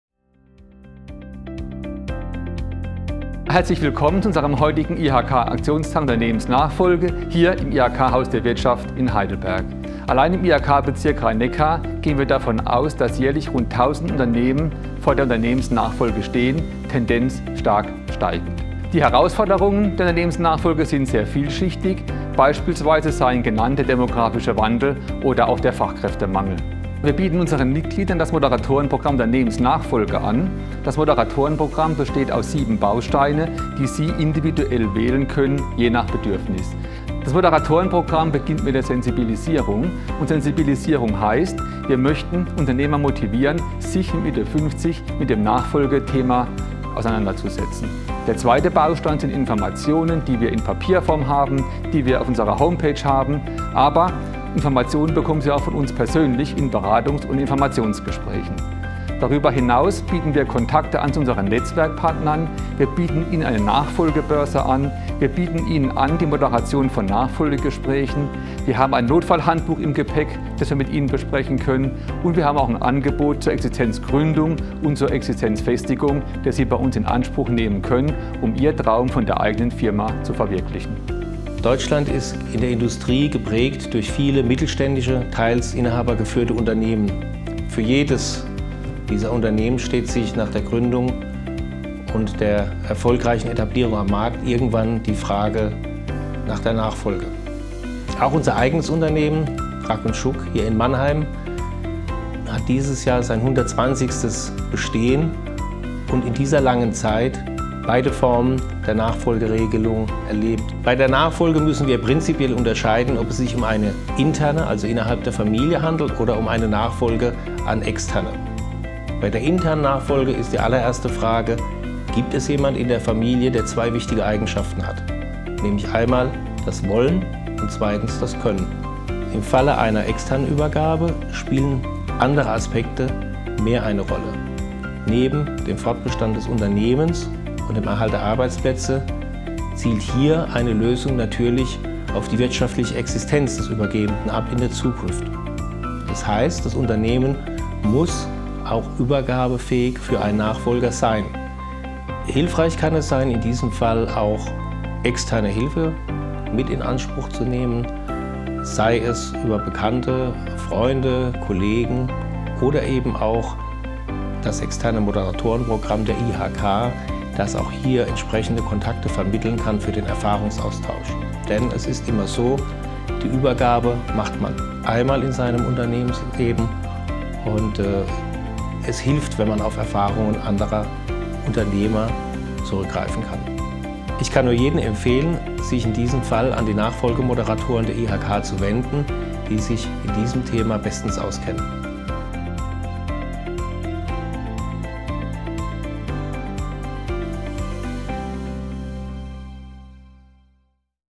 Moderatorenprogramm Unternehmensnachfolge
Die Unternehmensnachfolge zu regeln ist eine der größten Herausforderungen und emotionalsten Aufgaben in einem Unternehmerleben. Das Moderatorenprogramm Unternehmensnachfolge der IHK Rhein-Neckar bietet Übergebern wie auch potenziellen Übernehmern, wertvolle Unterstützung ihre Ziele zu erreichen.